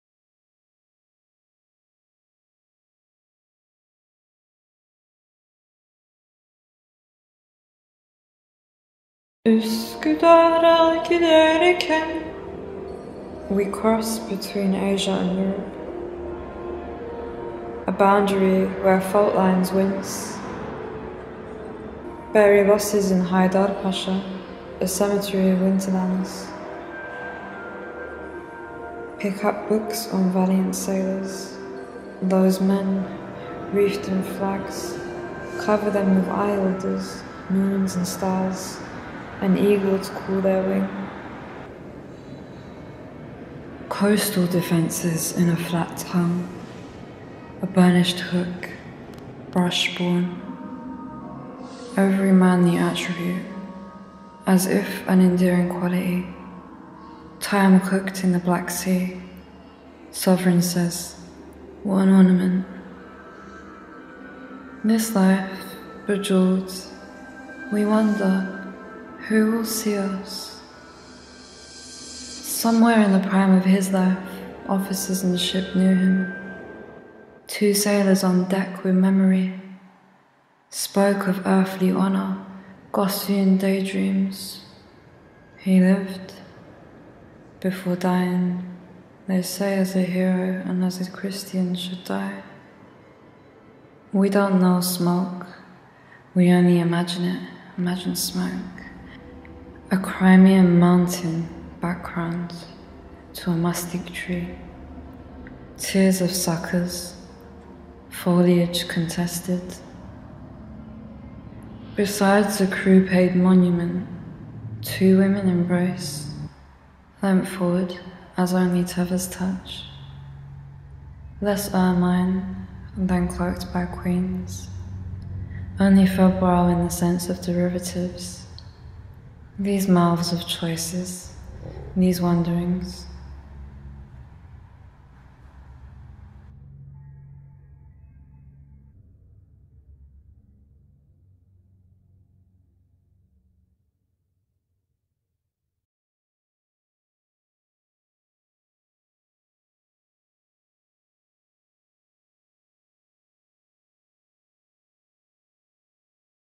'Resin Tears' by Tice Cin (50 Monuments in 50 Voices)
A Poem & Soundscape by poet, novelist and sound artist Tice Cinin in response to the monument to the 1855 monument  by sculptor Matthew Noble to Captain Edmund Moubray Lyons (1819–1855), mortally wounded at Sevastopol during the Crimean War, while in command of HMS Miranda.

The digital soundscape here was created from recordings made in St Paul's Cathedral.

For more details about this and the 50 Monuments in 50 Voices project,